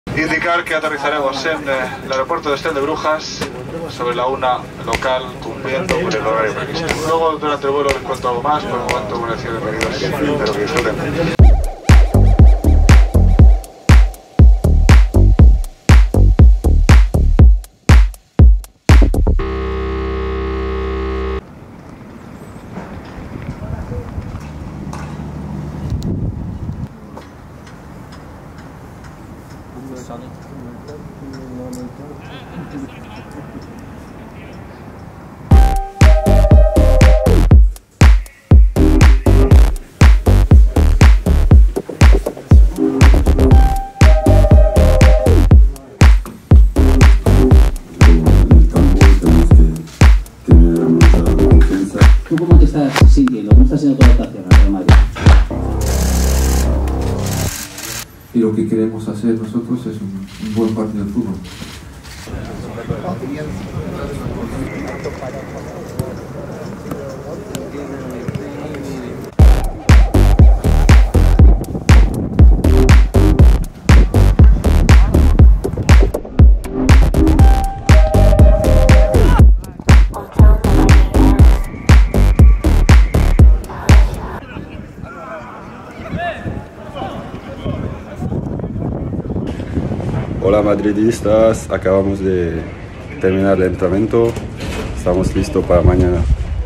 Champions League diaries | Club Brugge vs Real Madrid (Day One)
Go behind the scenes with us in Belgium and see Real Madrid's final preparations ahead of the final Champions League group stage game against Club Brugge!